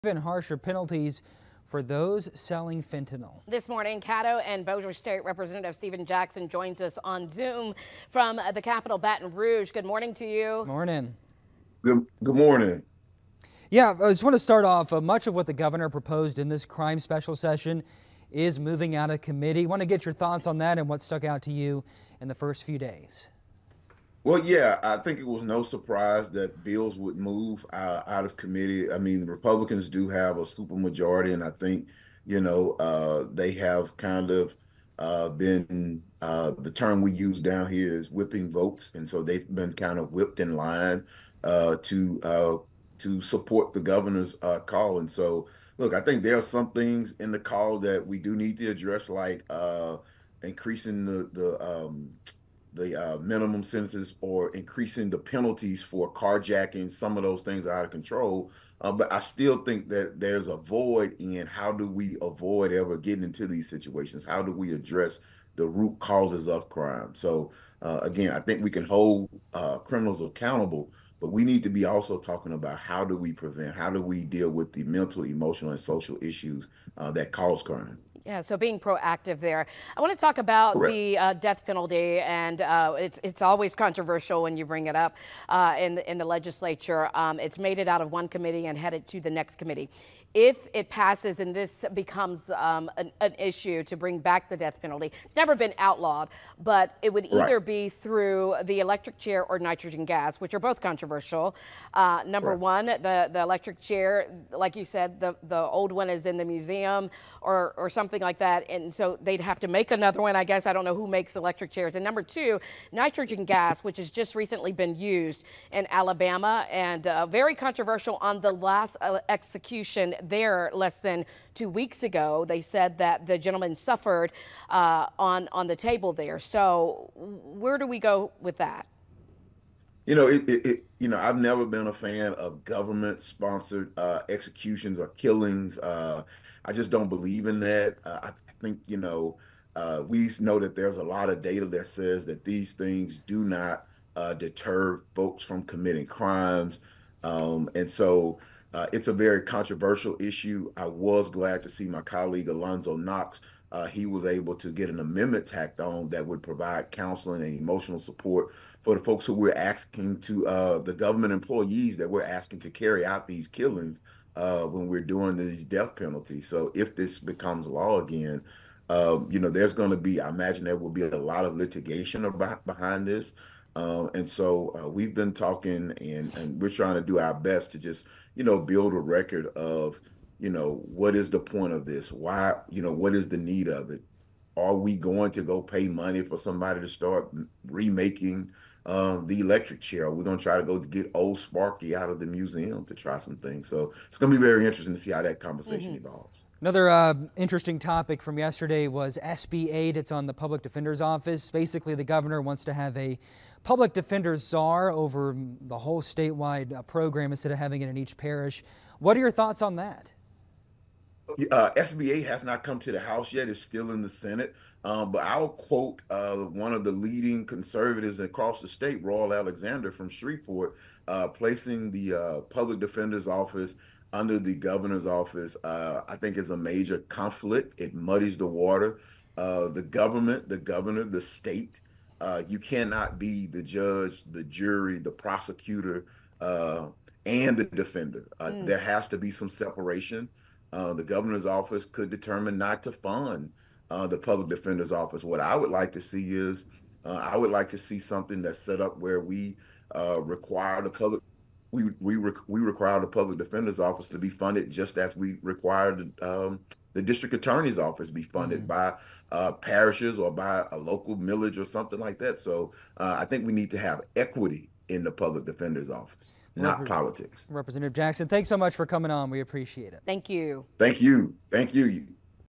Rep. Steven Jackson shares latest on crime special session from Baton Rouge
Discussion topics range from crime, the death penalty and rampant car jackings.